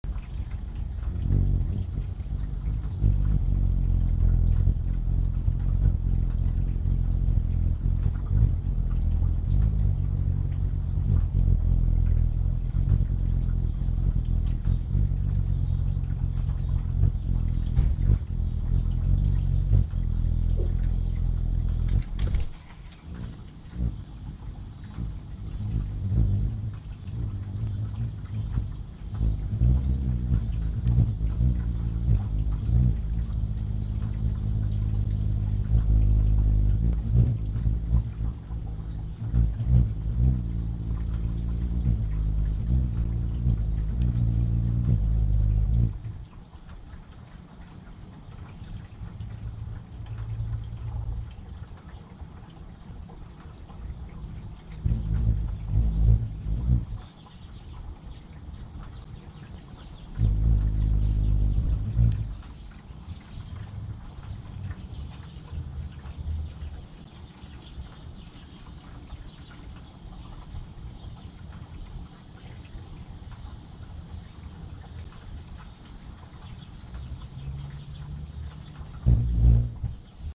Hummingbirds using Macro lens on my Gopro
This is my very first attempt to use my Macro lens on my Gopro hero6